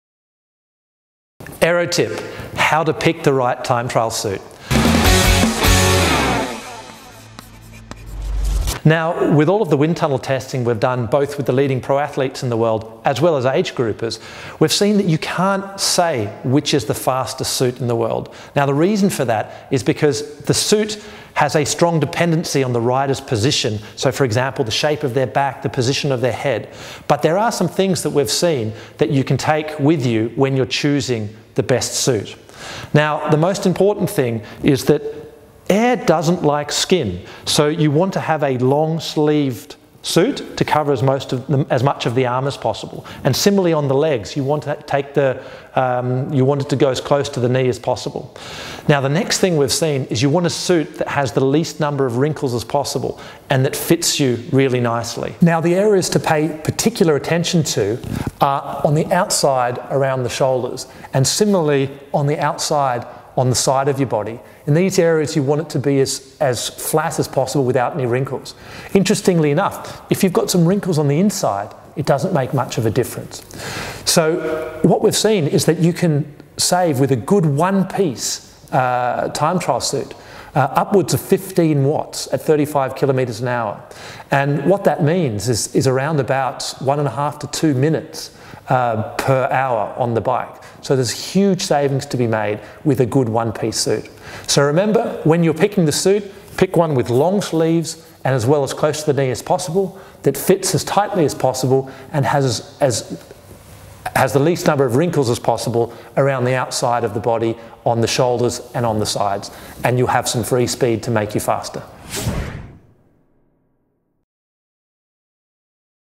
Swiss Side AERO TIP - TIME TRIAL APPAREL
Swiss Side's technical director, Jean-Paul Ballard talks about the effect of the time trial apparel on your aerodynamics. Every WATT counts! More information at swissside.com.